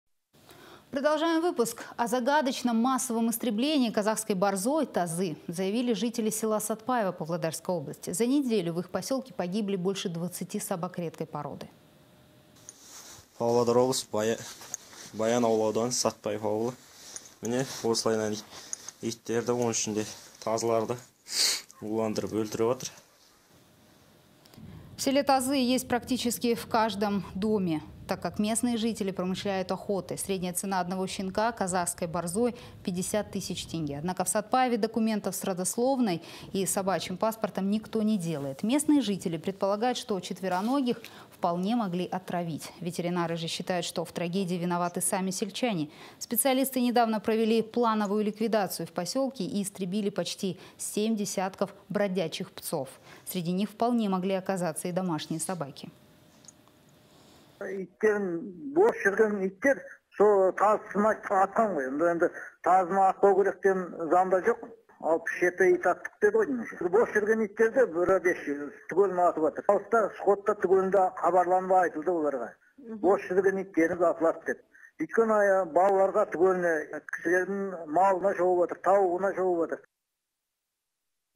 В Павлодарской области за неделю истребили более 20 собак редкой породы тазы
О загадочном массовом истреблении казахской борзой тазы заявили жители села имени Сатпаева Павлодарской области. За неделю в их поселке погибли больше двадцати собак редкой породы.
Баянаульский район, село имени Сатпаева. Вот так отравили и убили собак, среди которых есть и тазы. В селе тазы есть практически в каждом дворе, так как местные жители промышляют охотой. Cредняя цена одного щенка казахской борзой - 50 тысяч тенге. Однако в Сатпаеве документов с родословной и собачьим паспортом никто не делает.  Местные жители предполагают, что четвероногих вполне могли отравить. Ветеринары же считают, что в трагедии виноваты сами сельчане. Специалисты недавно провели плановую ликвидацию в поселке и истребили почти семь десятков бродячих псов. Среди них вполне могли оказаться и домашние собаки.